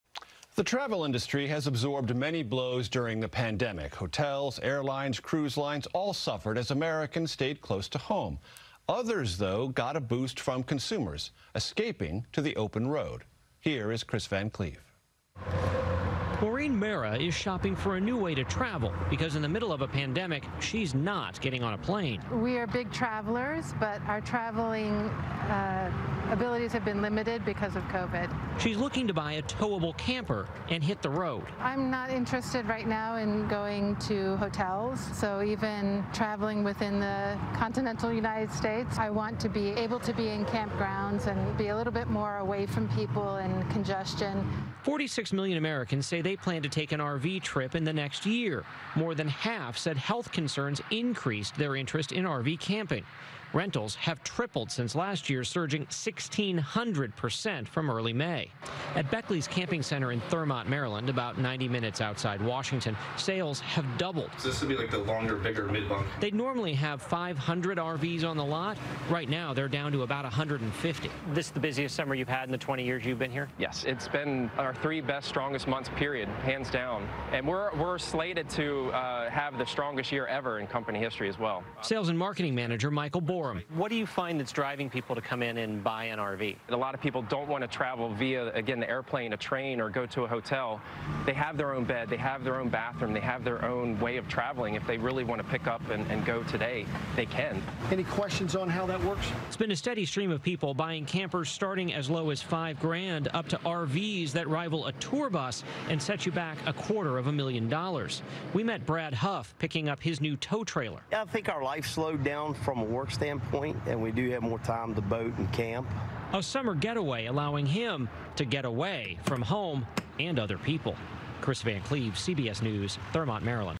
RV ridership surges as Americans find alternate means of travel amid coronavirus pandemic
As the coronavirus pandemic continues to deter travelers from flying, many Americans are finding other ways to satisfy their travel needs. Kris Van Cleave reports.